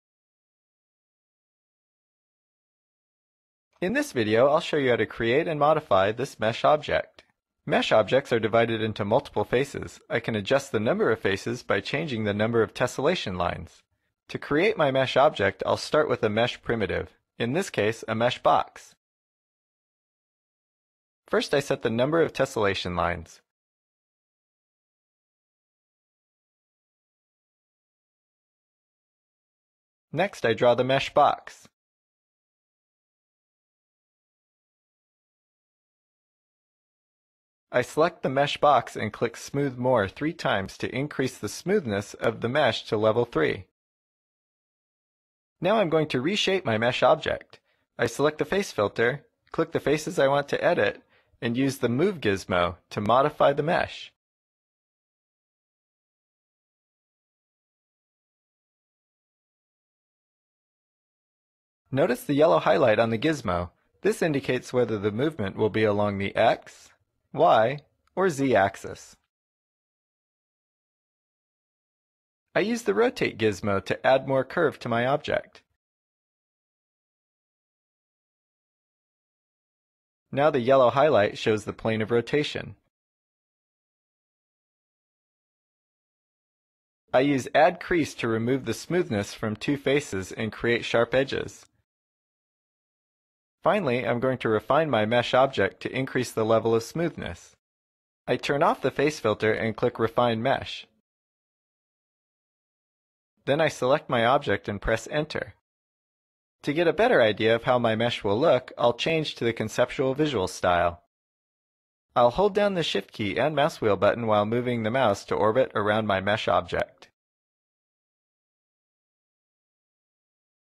Create and Modify 3D Meshes - AutoCAD 2011 - Getting Started Videos
3D objects in AutoCAD include lines, solids, and surfaces. Smooth surfaces can be defined and controlled with a mesh. This video provides an overview of creating and modifying 3D surface meshes.